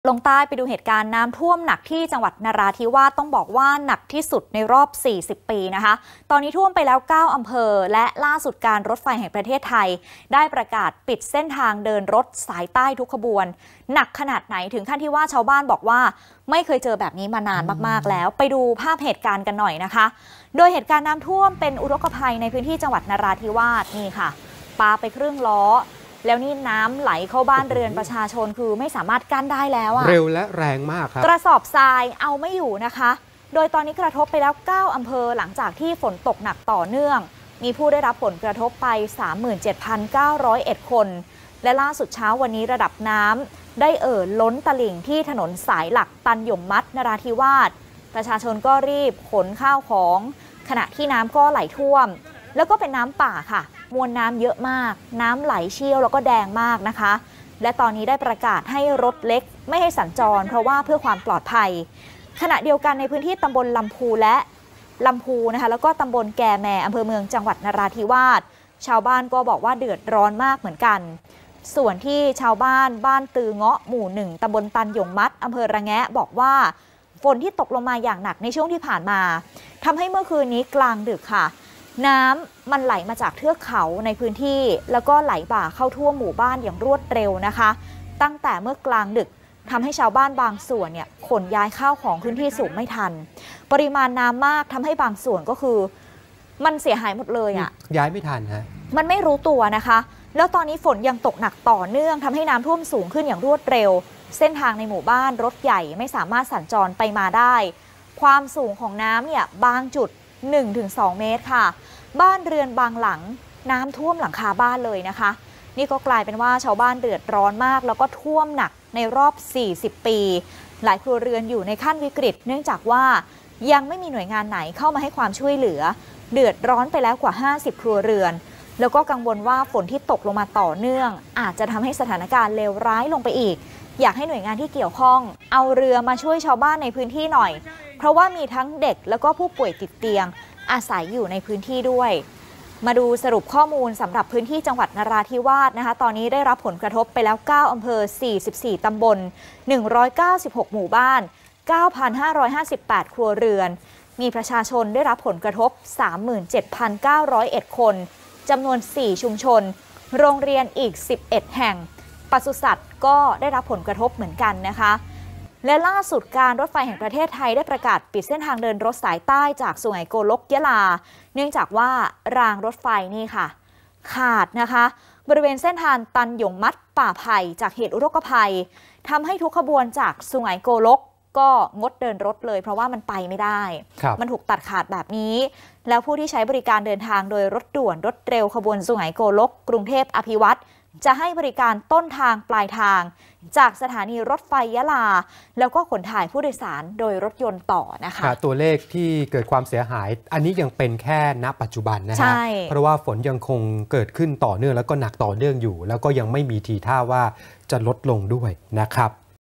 นราธิวาสน้ำท่วมหนักรอบ 40 ปี - ปิดเส้นทางเดินรถไฟสายใต้
น้ำท่วม จ.นราธิวาส หนักสุดในรอบ 40 ปี ท่วมแล้ว 9 อำเภอ ล่าสุดการรถไฟแห่งประเทศไทย ประกาศปิดเส้นทางเดินรถสายใต้ทุกขบวน